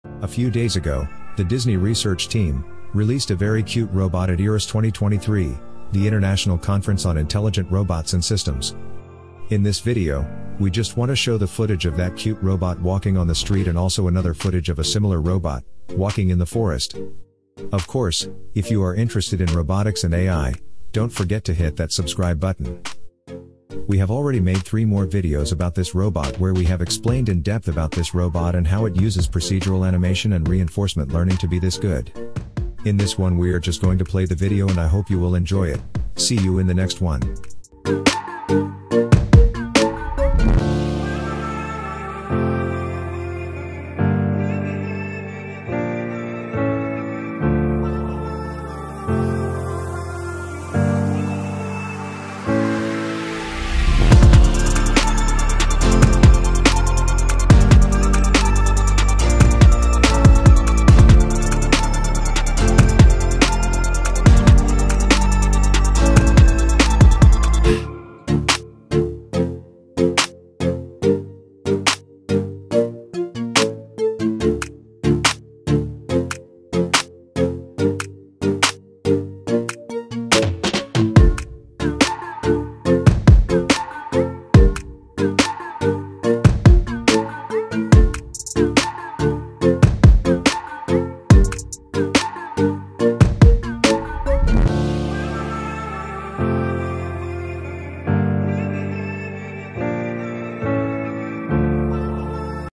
DROIDS From Disney Robots Walking in Galaxy Edge: A Star Wars Robot
Have you ever wondered what it would be like to see a cartoon character come to life? Well, wonder no more, because Disney has created a new robot that walks like a cartoon character!

This robot is not just a toy, but a sophisticated bipedal machine that can balance dynamically, navigate different terrains, and express emotions through body language. It was developed by a team of Disney Research scientists and animators, who presented it at the 2023 IEEE/RSJ International Conference on Intelligent Robots and Systems (IROS) in Detroit.

In this video, you will see the robot walking in the street and in the forest, showing off its amazing abilities and personality. You will also learn how the robot was designed, how it works, and what it can do.

This robot is part of Disney's efforts to bring animation into the real world and to create robots that can interact with humans in natural and engaging ways. It is also a demonstration of how art and science can work together to create something magical.

If you are a fan of Disney, robotics, or both, you will love this video. Watch it now and be amazed by Disney's new robot!